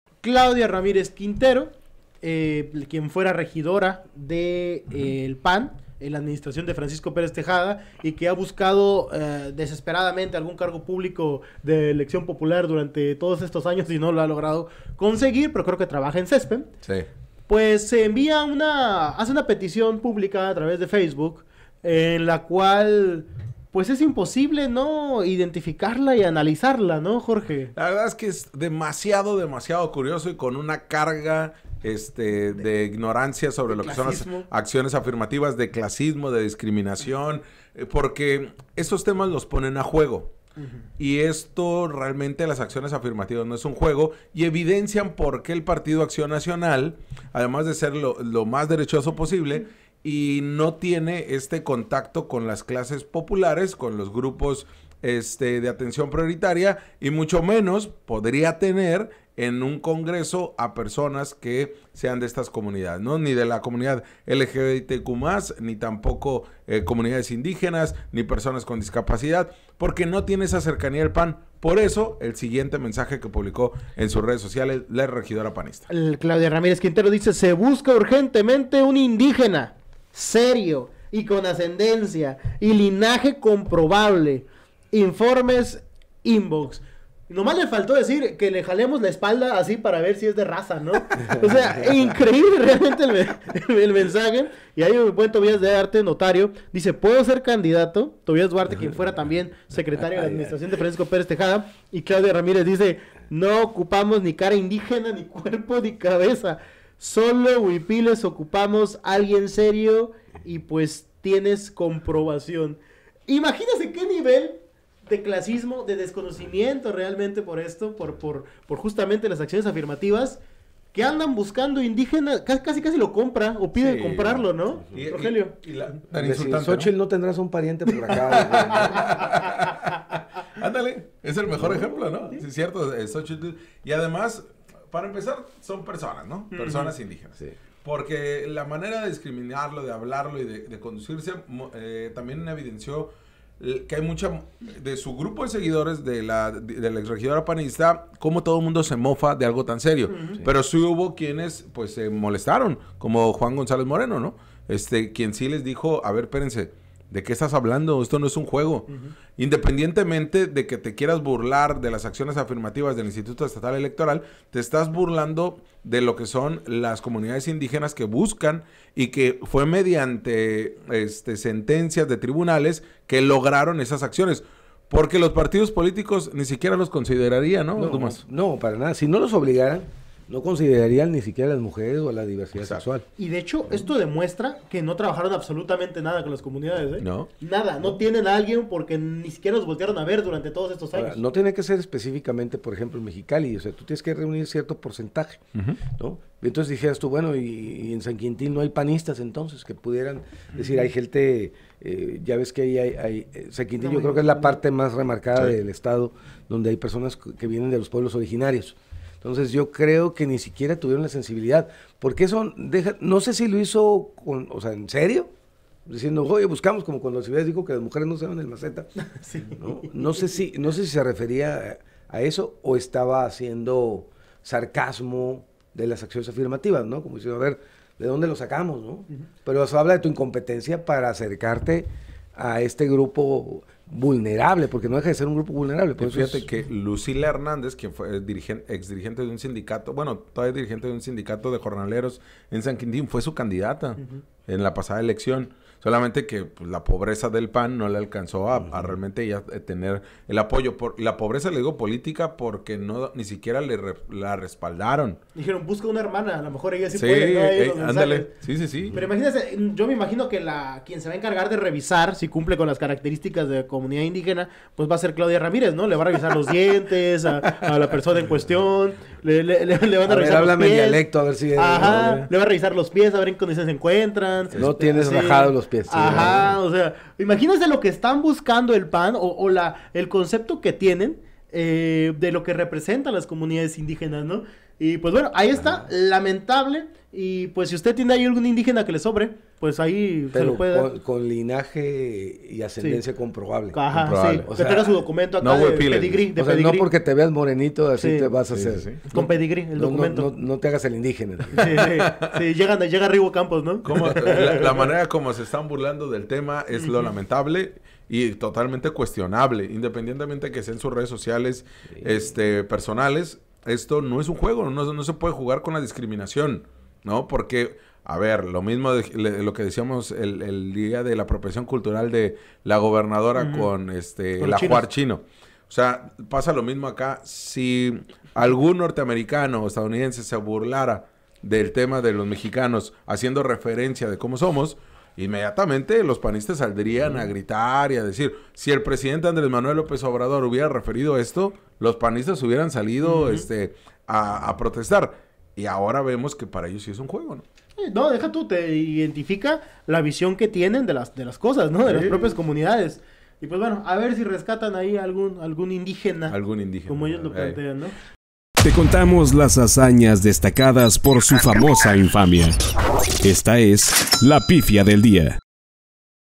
Si alguien tiene un “indígena” que le sobre, el PAN lo anda buscando hasta por Marketplace
Si alguien tiene un “indígena” que le sobre, el PAN lo anda buscando hasta por Marketplace: En esta sección, exploramos una curiosa búsqueda del PAN. ¿Por qué están buscando un “indígena” en lugares inesperados? 🤔🔔

📰 EnVivo de Lunes a Viernes a las 8am en Facebook y Youtube Live, ¿Te perdiste de algo? Encuéntranos acá

Ciudad Capital se emite en vivo desde @casacreala en Mexicali